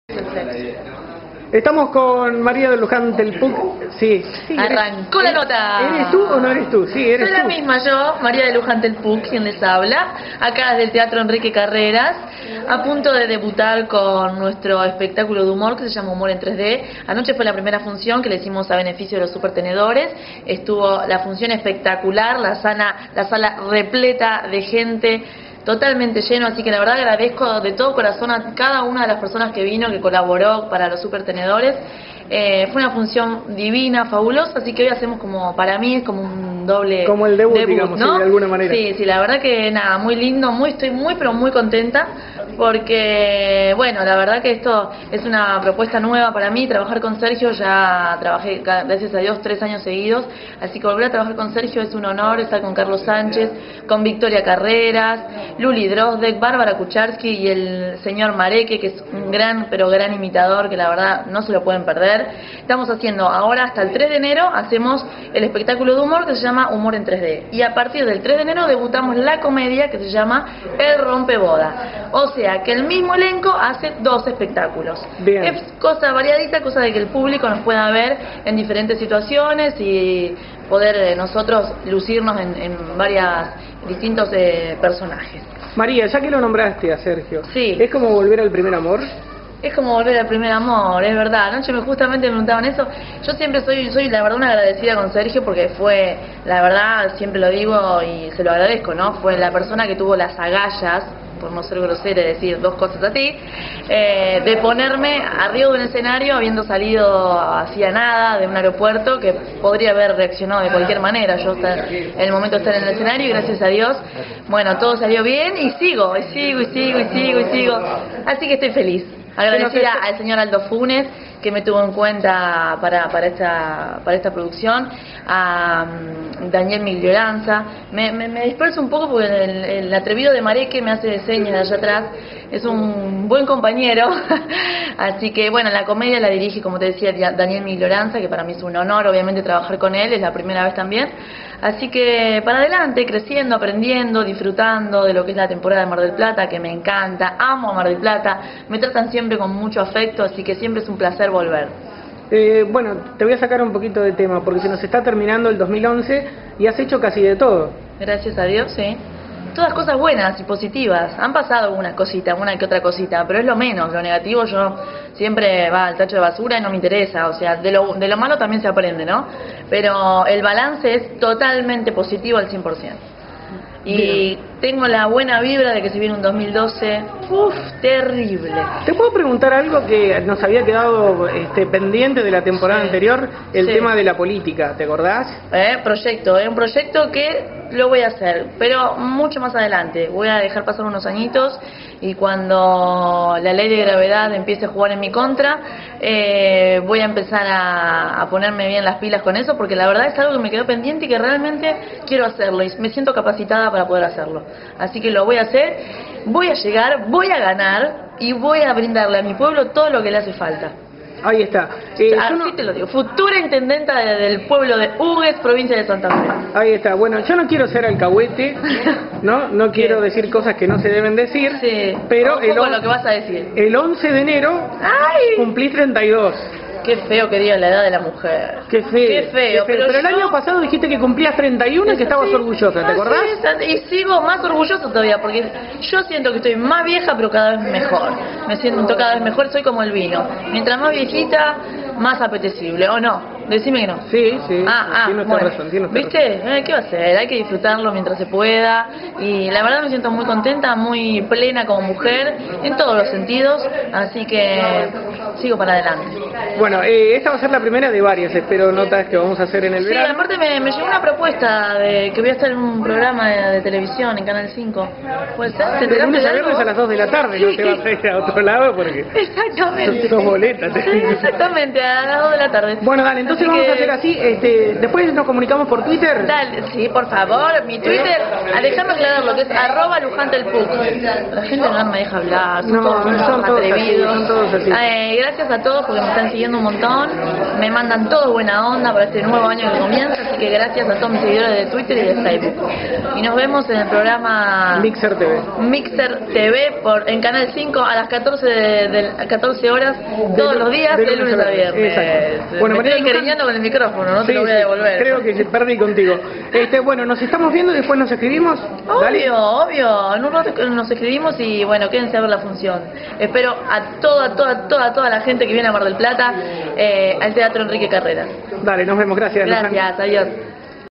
telpuk.mpg
nota k realizamos en la puerta del tratro Enrique Carreras para mixer tv